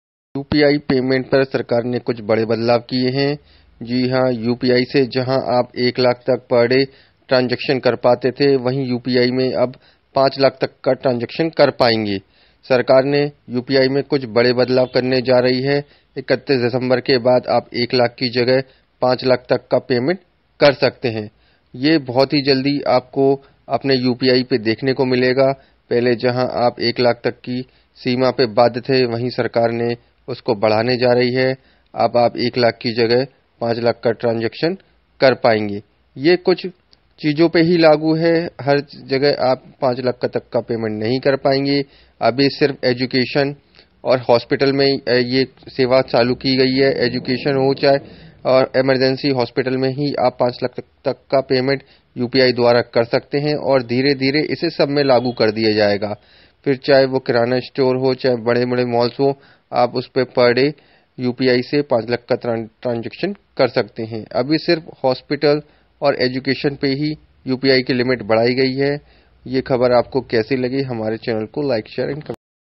UPI से अब 5 लाख तक की पेमेंट कर पाएंगे।
agar aap upi use karte hain to upi ki daily limit ek lakh Thi ab vah Badhkar 5 lakh Hone Wali Hai Jahan aap 1 lakh ka payment upi Se Karte The ab 5 lakh Tak ka payment ab kisi ko bhi kar Payenge yah Sarkar Ne yah Faisla Lekar bahut bada Tohfa Diya Hai Aam aadmiyon ke liye